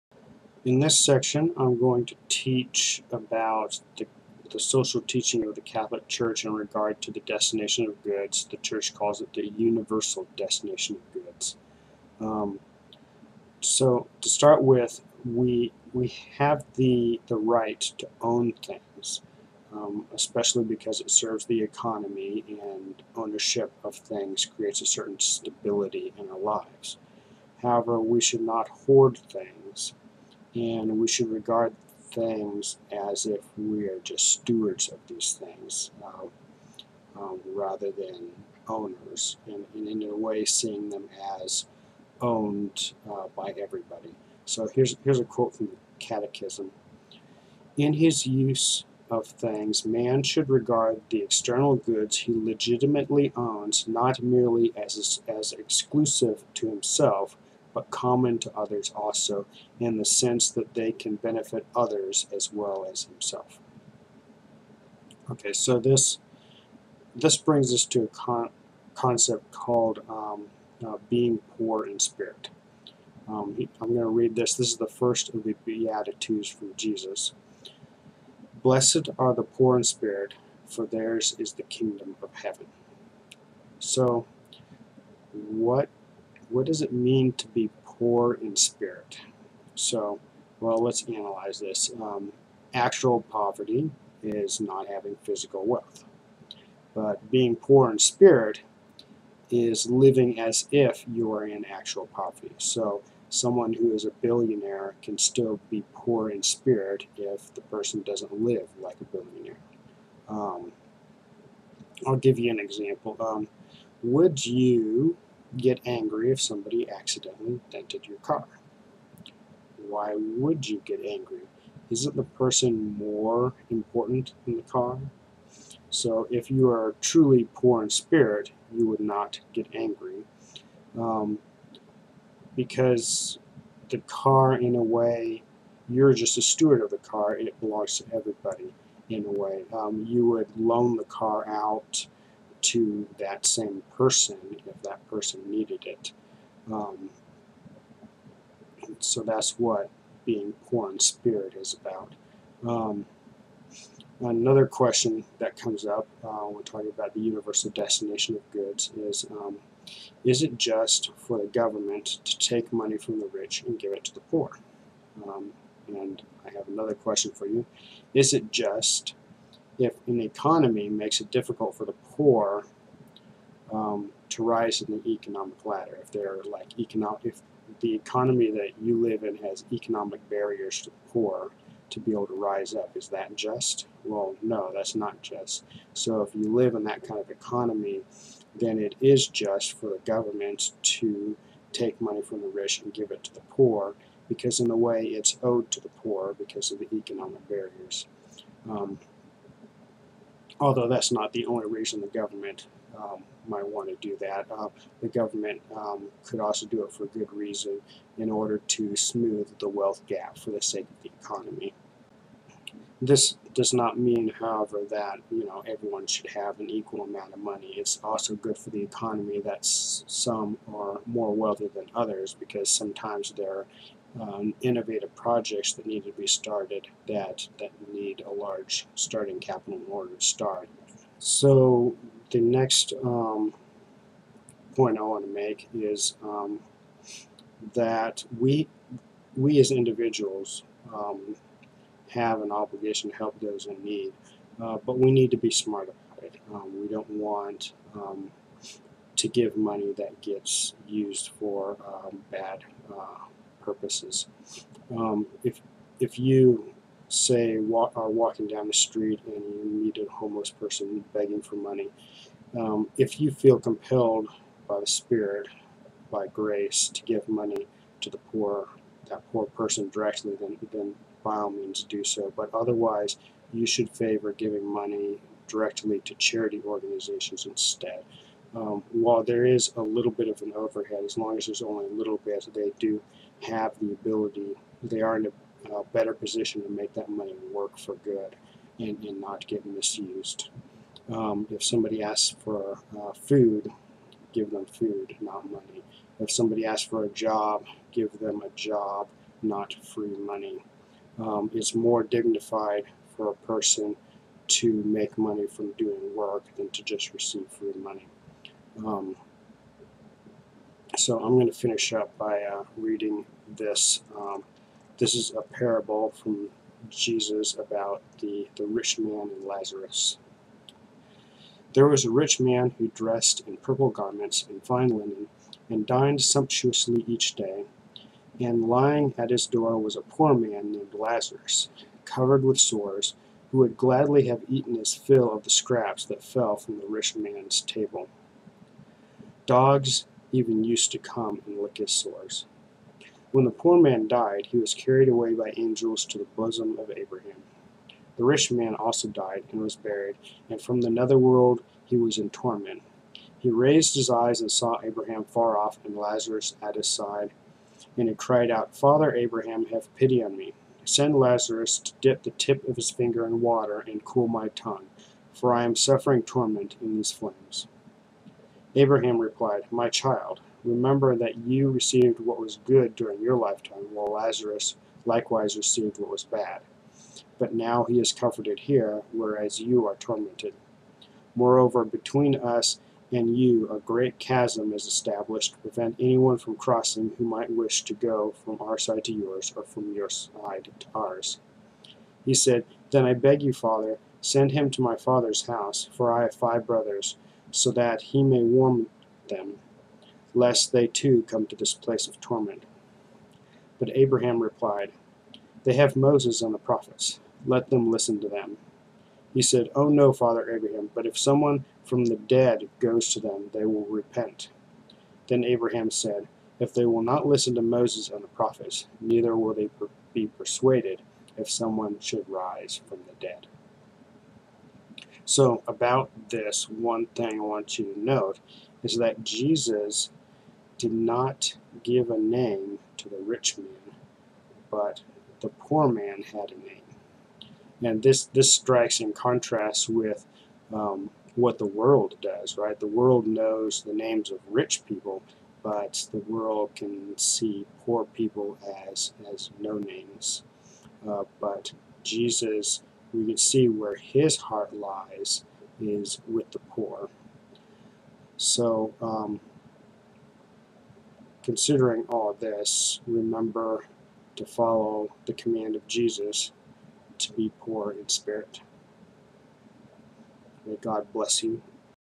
Social Teaching of the Church part 2 of 2
Universal destination of goods
Poor in spirit
Distribution of goods
Being smart about helping people
Parable of the rich man and Lazarus

Credits:
All Bible quotes are from the New American Bible, Revised Edition,